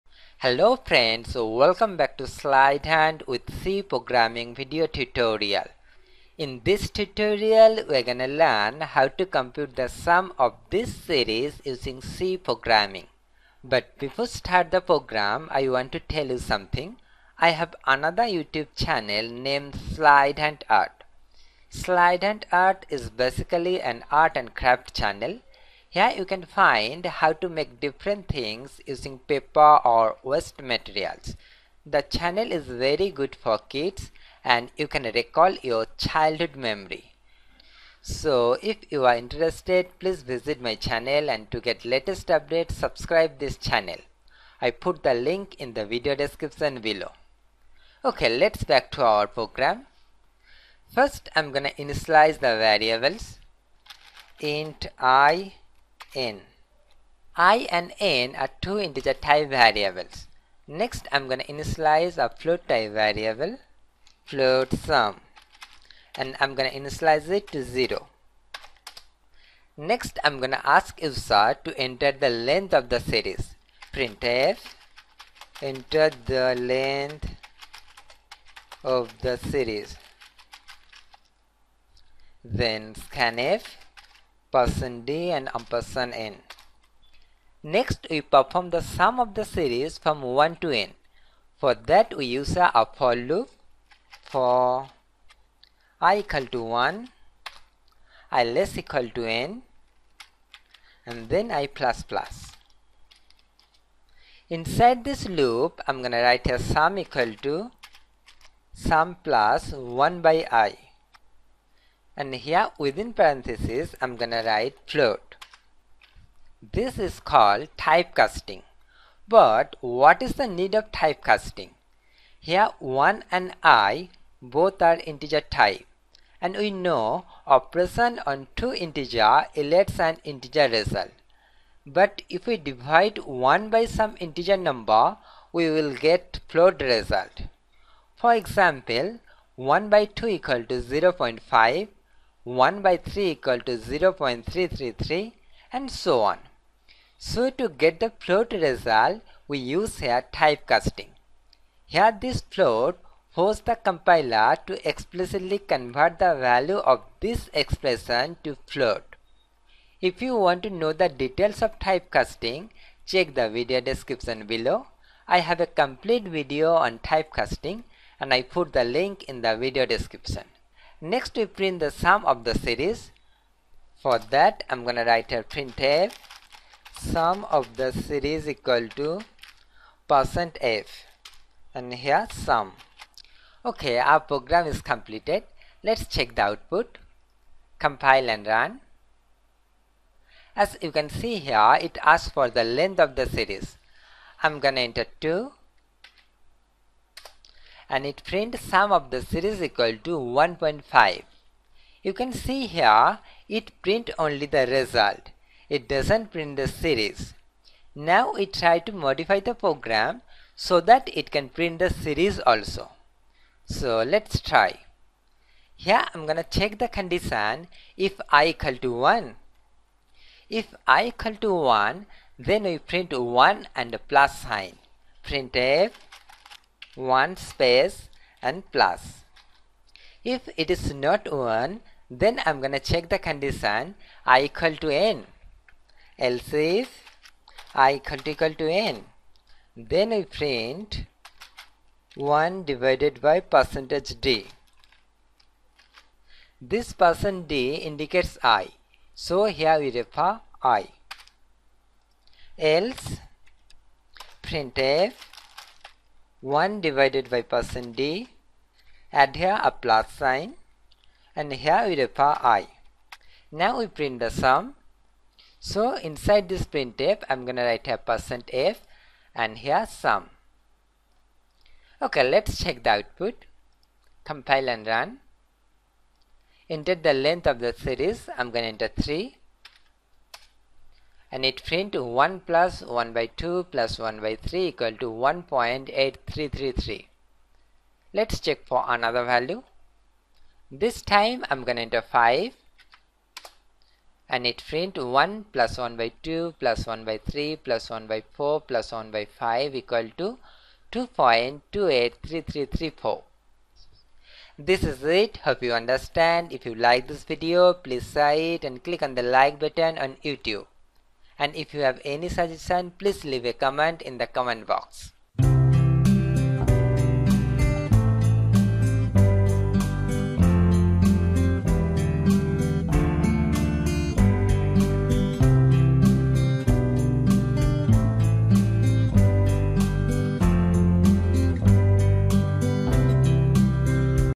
Compute sum of series in C programming | My other channel
In this tutorial I am going to show you how to compute the sum of this series 1+1/2+1/3+. +1/n in C programming. Here i show you step by step to calculate the sum of the series in C programming.